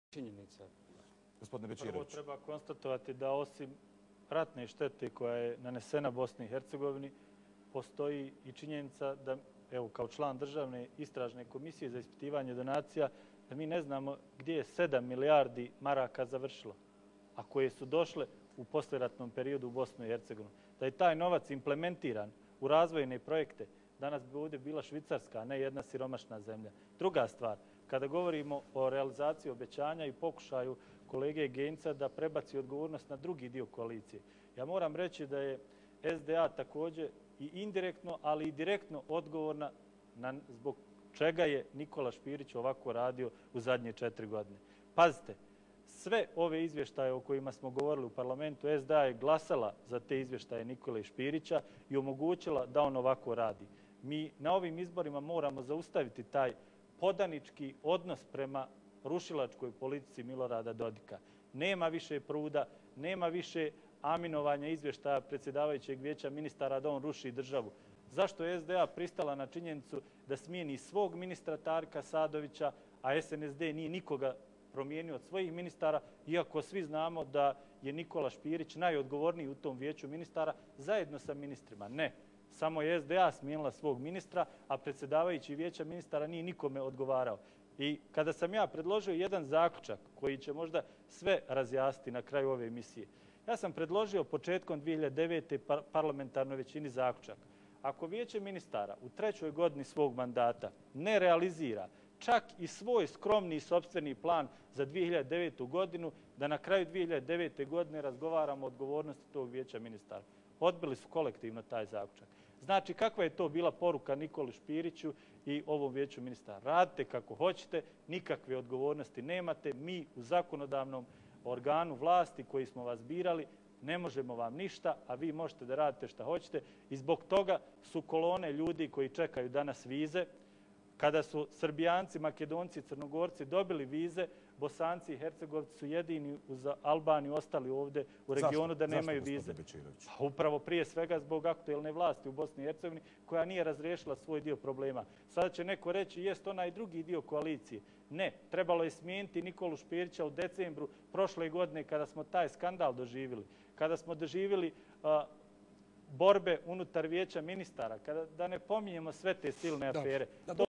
SDP BiH Hayat TV Denis Becirovic.flv
Denis Becirovic, debatna emisija o izborima 2010.